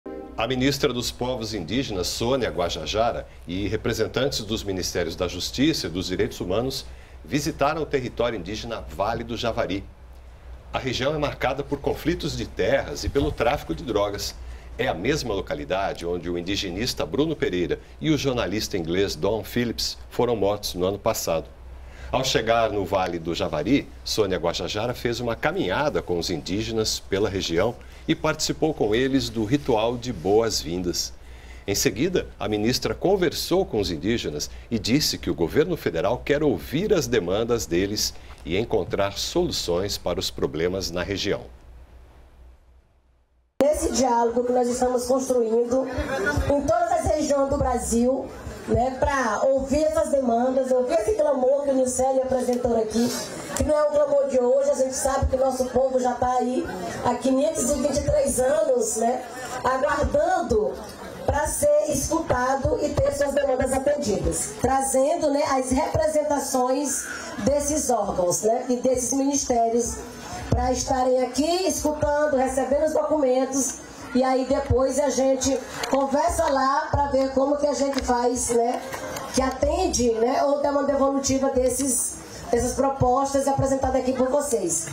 SONIA GUAJAJARA VISITA TERRITÓRIO INDÍGENA NO VALE DO JAVARI
A ministra dos Povos Indígenas, Sonia Guajajara, visitou o território indígena do Vale do Javari acompanhada de representantes de outros ministérios. A região é marcada por conflito de terras e tráfico de drogas.